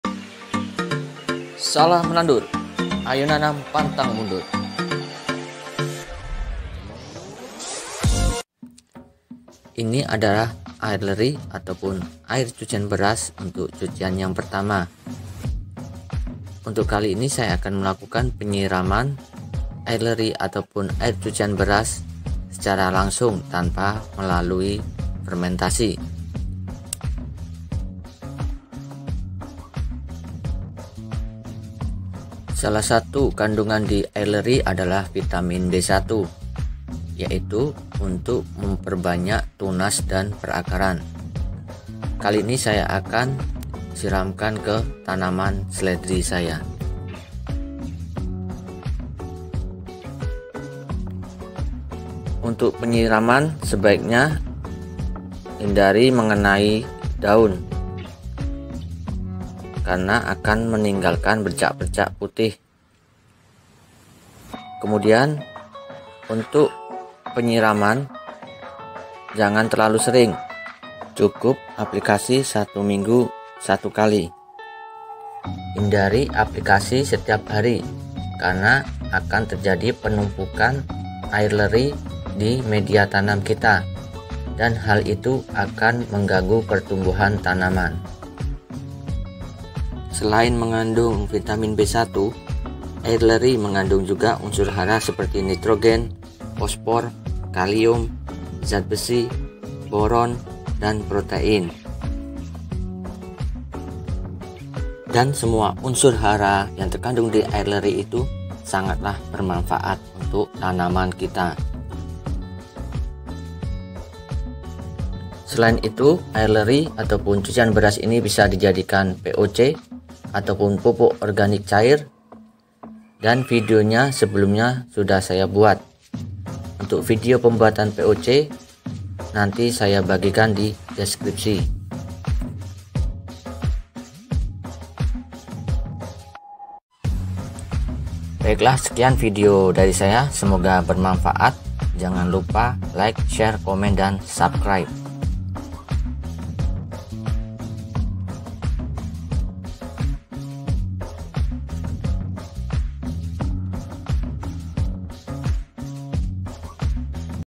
air cucian beras untuk tanaman(how use rice water for you plant), aplikasi dan kandungan di dalamnya
how use rice water for your plant/organic fertilizer from rice water
air  cucian beras mengandung unsur yang baik untuk tanaman .. contohnya vitamin B1 yang bermanfaat untuk Tunas dan akar